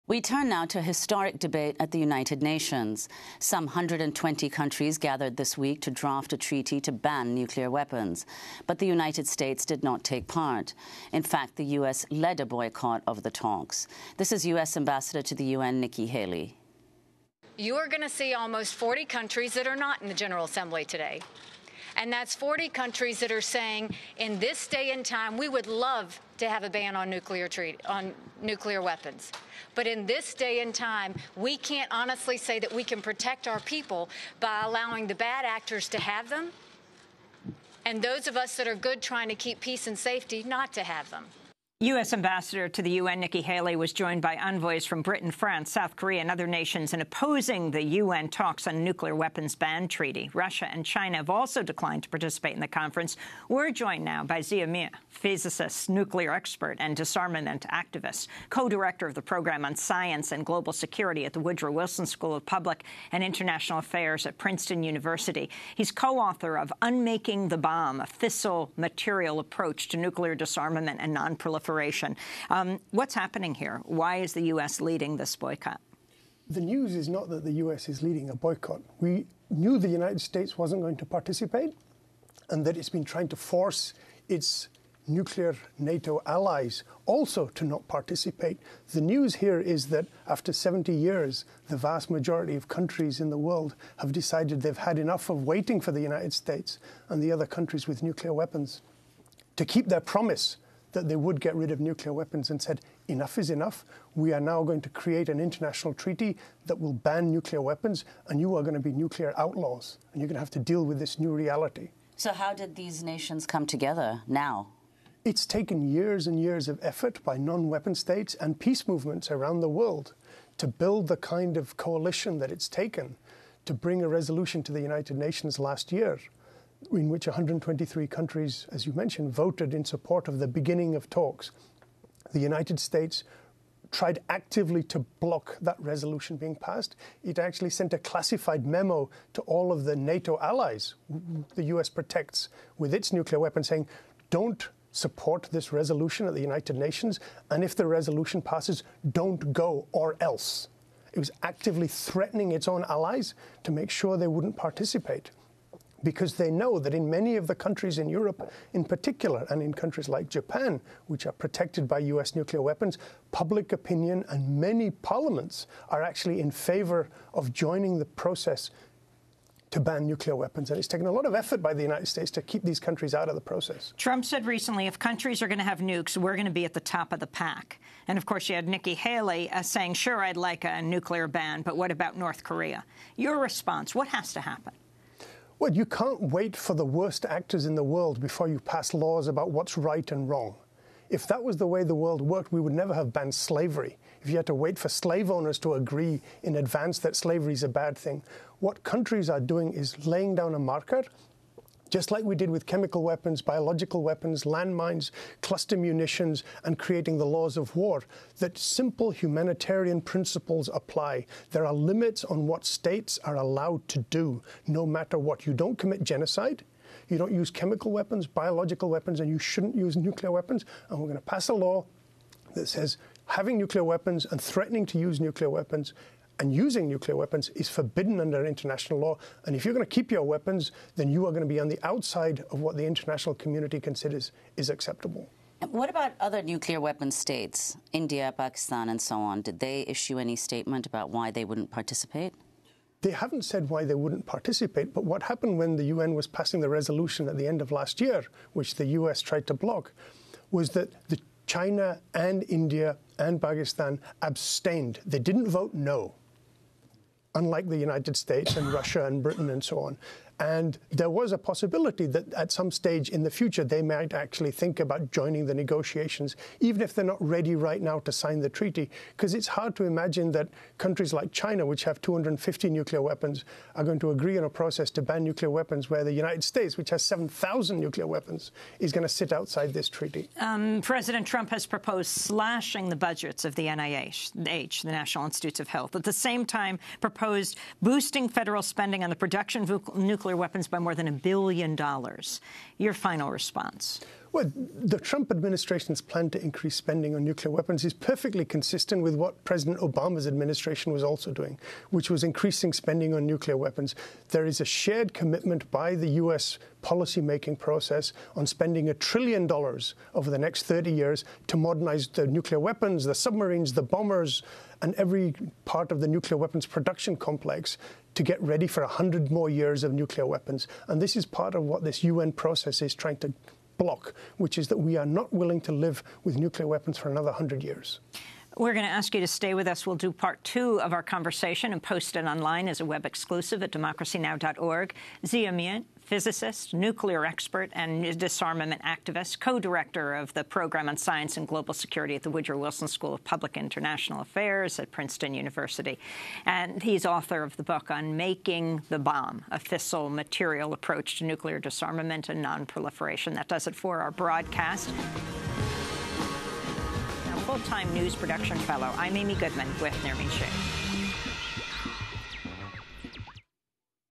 UN Considers a Historic Ban on Nuclear Weapons, But US Leads Boycott of the Talks
Some 120 countries gathered at the United Nations in March 2017 to draft a treaty to ban nuclear weapons. But the United States is leading a boycott of the talks. Meanwhile, more than 2,000 scientists signed an open letter endorsing the U.N. talks, and, on Tuesday, Pope Francis encouraged the United Nations to pursue the "total elimination" of nuclear weapons. We speak to Zia Mian, physicist, nuclear expert and disarmament activist. He is co-director of the Program on Science and Global Security at the Woodrow Wilson School of Public and International Affairs, Princeton University.

Dr. Zia Mian is a physicist, nuclear expert and disarmament activist. He is co-director of the Program on Science and Global Security at the Woodrow Wilson School of Public and International Affairs, Princeton University. Mian is co-author of Unmaking the Bomb: A Fissile Material Approach to Nuclear Disarmament and Nonproliferation.